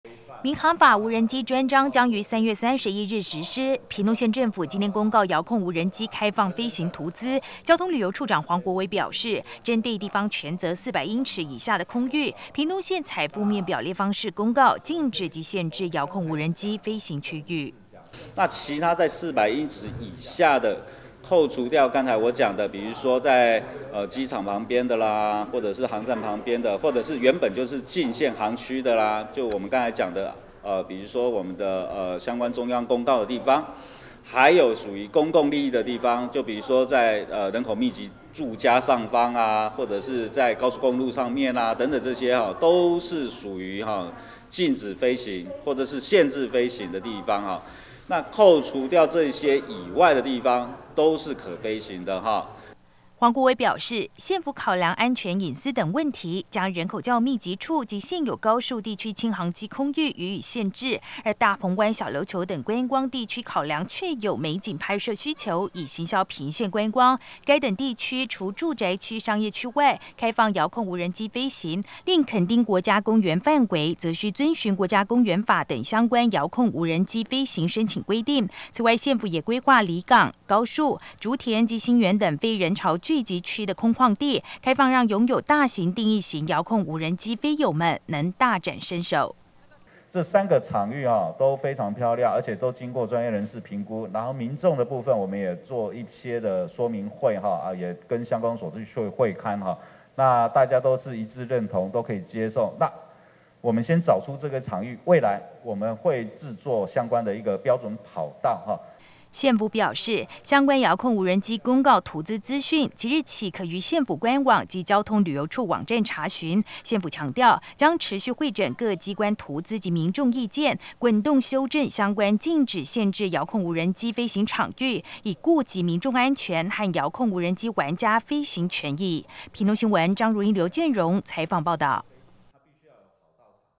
109 0324 屏縣遙控無人機空域 採負面表列禁限飛場
因應民用航空法遙控無人機專章將於3月31日施行，屏東縣政府今天針對全縣遙控無人機開放飛行區域，以負面表列方式公告屏縣禁止及限制遙控無人機飛行場域，並就都市計畫區如住宅區等人潮較多處規劃予以限制，另考量較大型的定翼型遙控無人機實際場地操作需求，將規劃里港、高樹、竹田、新園等近河濱區域作為飛行場域，未來也將持續滾動修正。